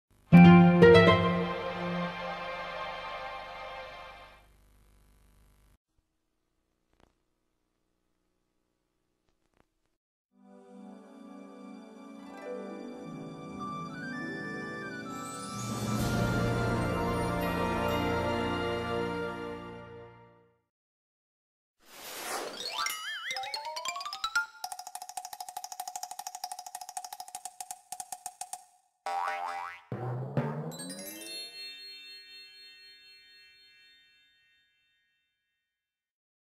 Sony Wonder Logo History Simplified
The children's home video arm of the major American film company's home video arm Sony Pictures Home Entertainment. They distributed various children's shows to VHS and DVD, including Nickelodeon and Sesame Workshop. Based in New York City.
0:00 Signation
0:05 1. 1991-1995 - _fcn
0:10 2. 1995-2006, 2014-present - 4R Geo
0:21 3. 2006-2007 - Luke